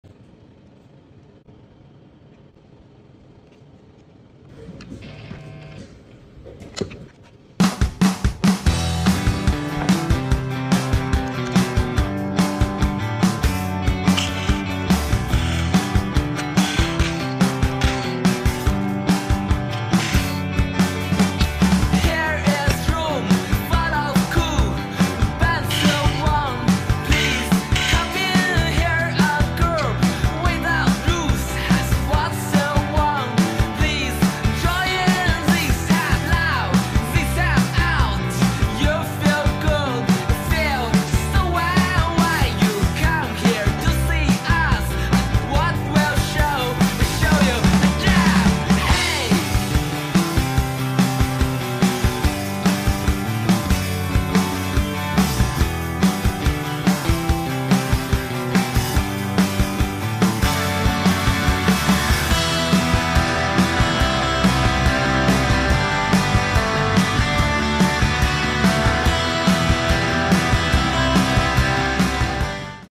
Diving into the secret of game controller...
This is a disassemble video of EasySMX 9124 Pro controller. In this video, you can take a closer look of the inside of a game controller and what Hall effect joysticks & triggers look like.